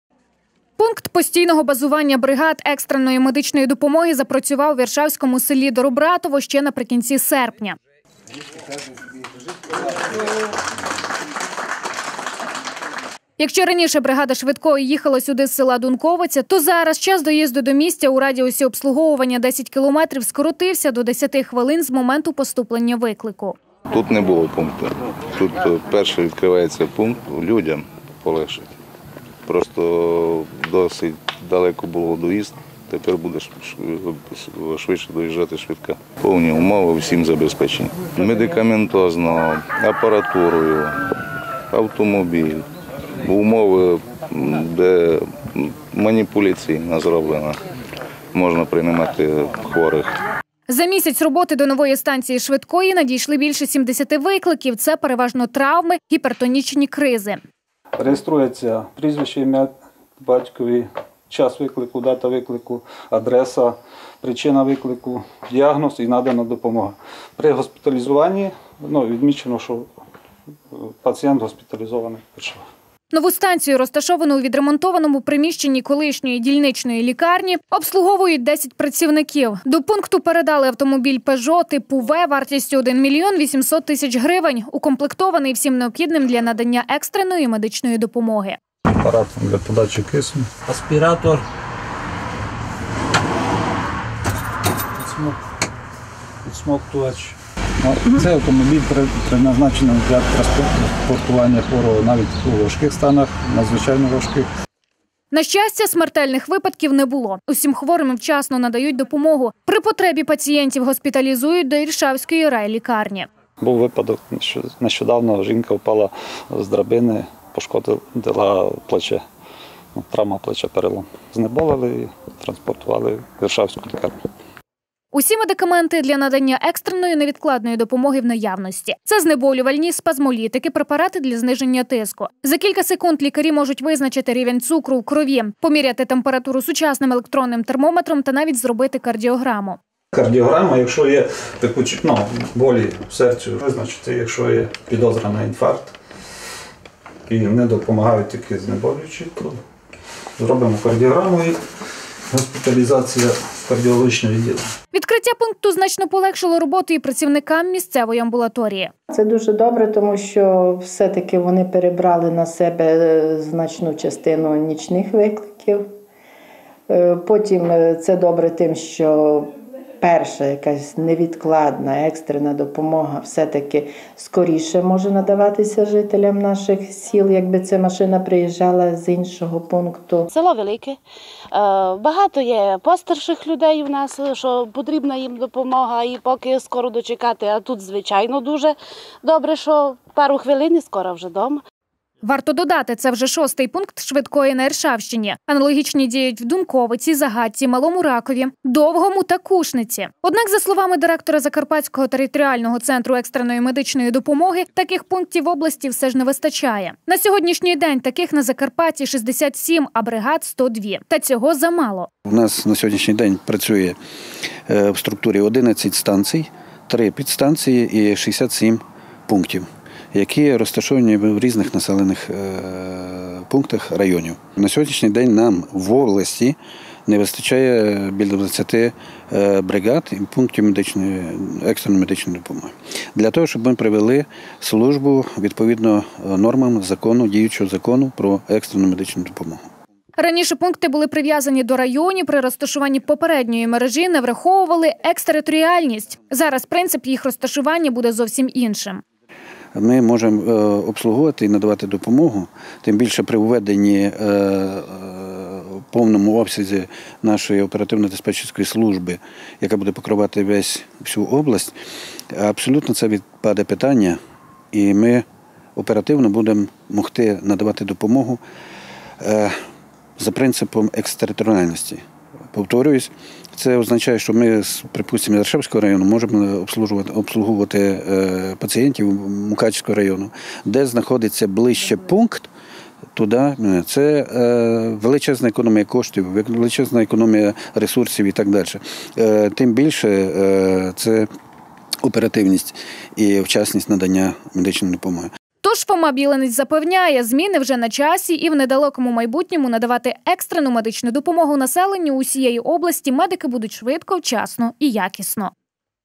Нова станція "швидкої"
Хоч пунктів екстреної медичної допомоги в області 67, та цієї кількості замало, щоб обслуговувати населення Закарпаття. Та зміни вже на часі, кажуть в Закарпатському територіальному центрі екстреної медичної допомоги. Нову мережу вже почали будувати, і роблять це, враховуючи екстериторіальність.